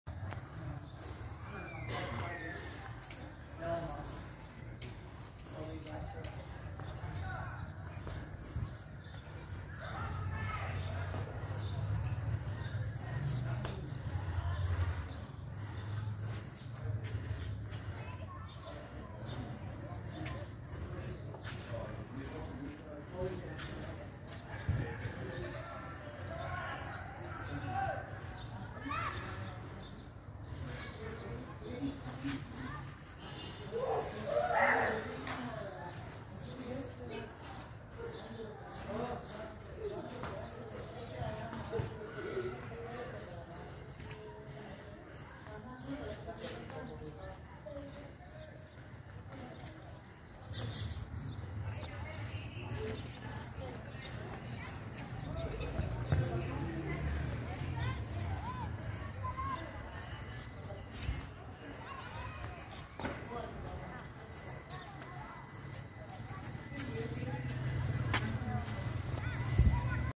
BEAUTIFUL VIEW OF TOCK,CITY. BEAUTY OF PUNJAB, PAKISTÁN.
BEAUTIFUL VIEW'S OF ATTOCK CITY FROM OUR ROOF TOP.AMAZING PLACE TO LIVE.